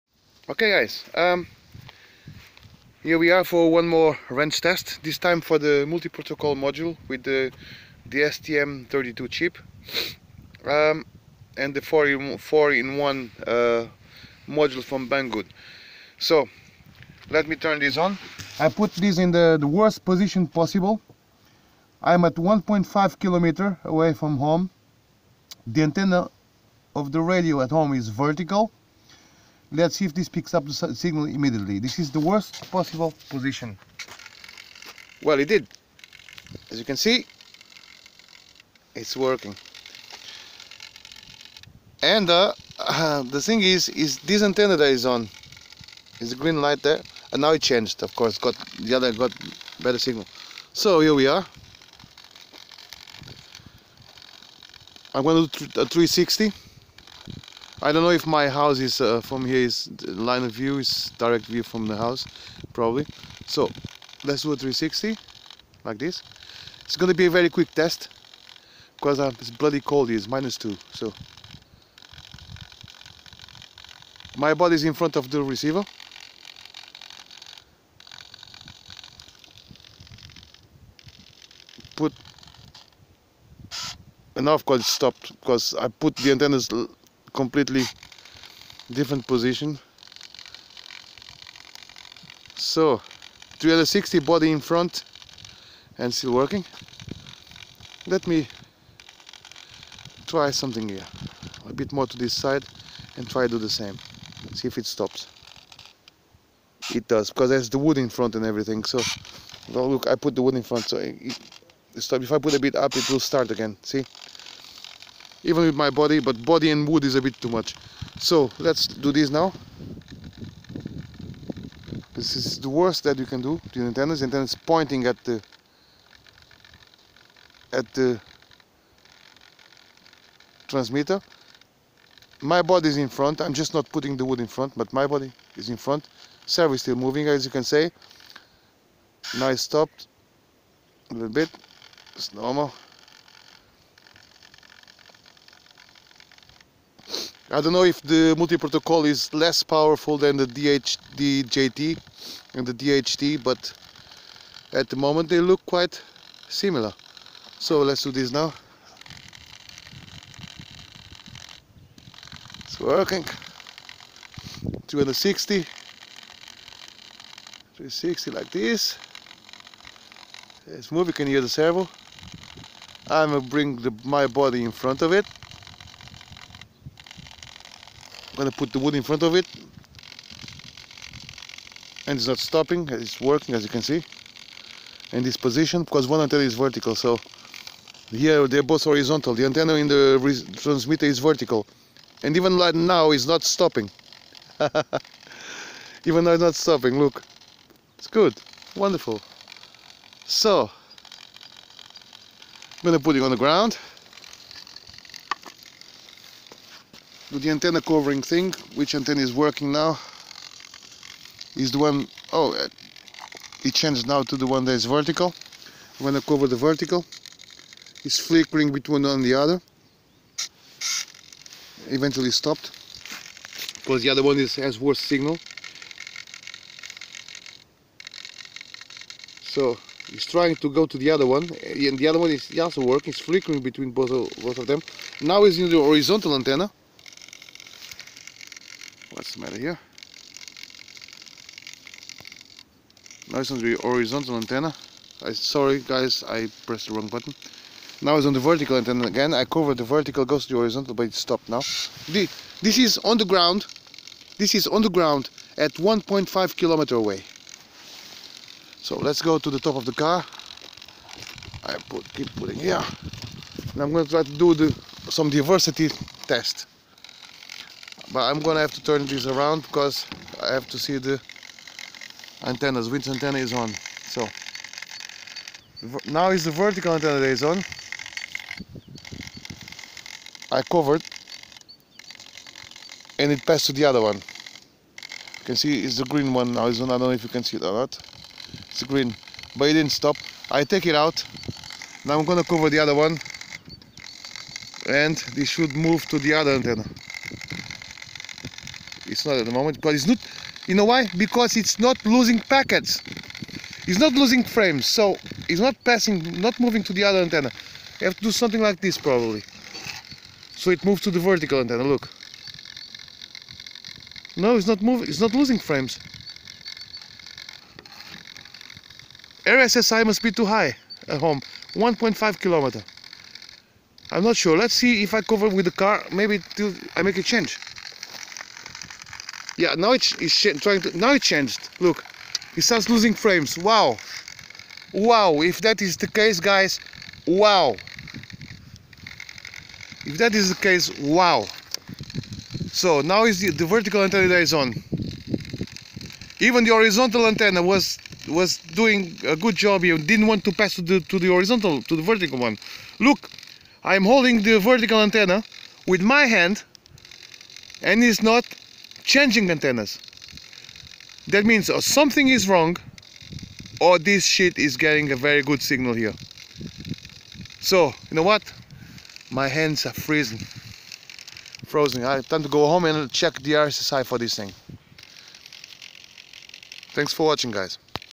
Range Test Multiprotocol Module and DIY Frsky_RX at 1.5Km
DIY multiprotocol Module with STM32 processor and 4 in 1 RF module from Banggood

DIY Frsky_RX with diversity and Dipole Antennas